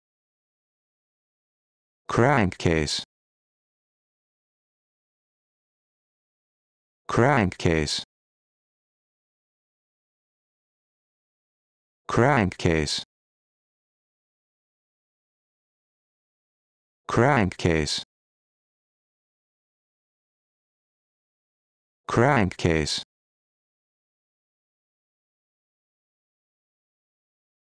How to Pronounce Crankcase
This video shows you how to pronounce Crankcase